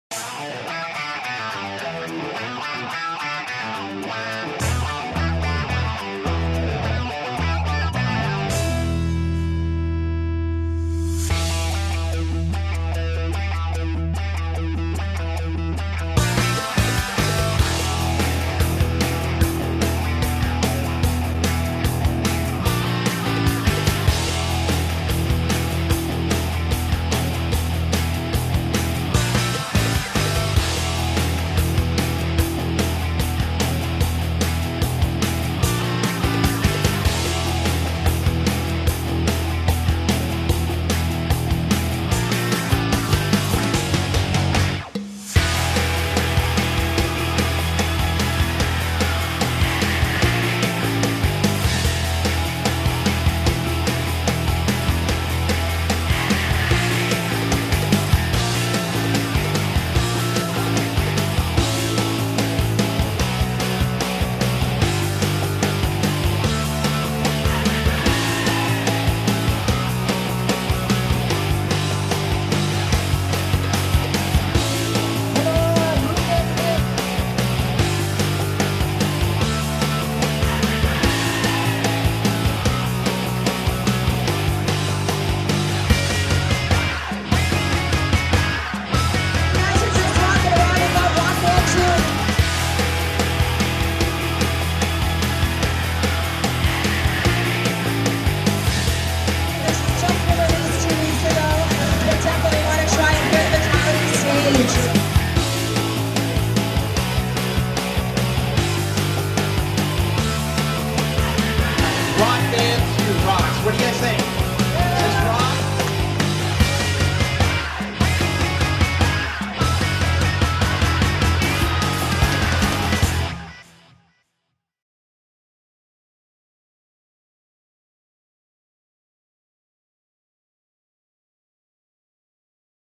Fatal1ty Booth at E for All 2008
Check out what the Fatal1ty Gaming Gear booth had to offer at the 2008 E for All Expo...

-Fatal1ty Quake 4 shootouts featuring the infamous Handicap Wheel.
-The Fatal1ty Obstacle Course: a first of its kind.
-Rock Band 2 on the main stage.